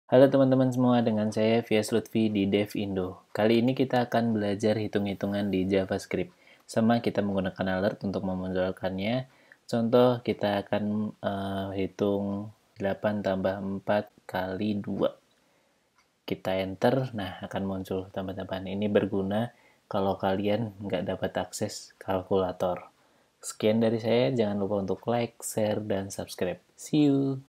Javascript Basic - Berhitung
Pada video kali ini saya menjelaskan bagaimana memanfaatkan javascript sebagai kalkulator sederhana